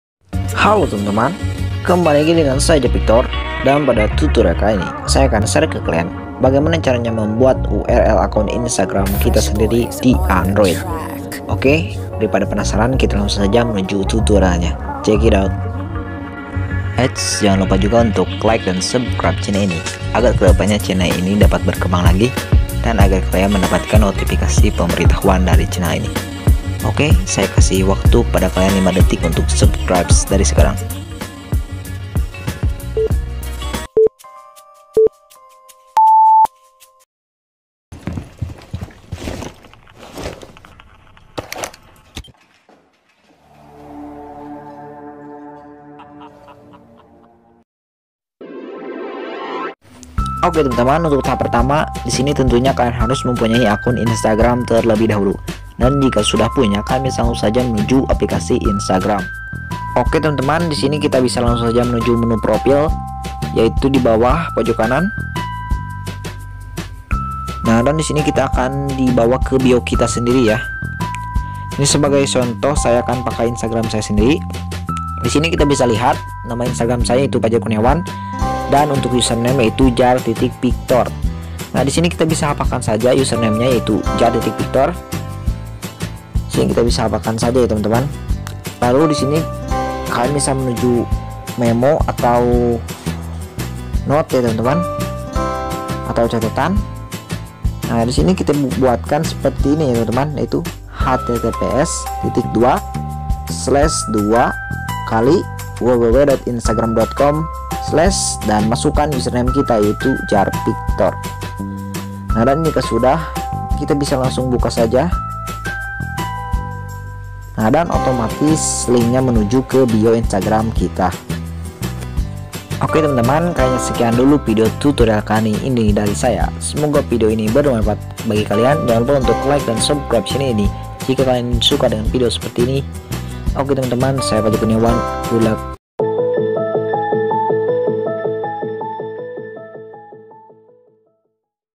Cara Membuat Link URL Instagram Sendiri
Hallo,teman-teman kembali lagi dengan saya zar victor.
Dan pada tutorial kali ini saya akan share ke kalian bagaimana caranya buat link url akun instagram kita di android.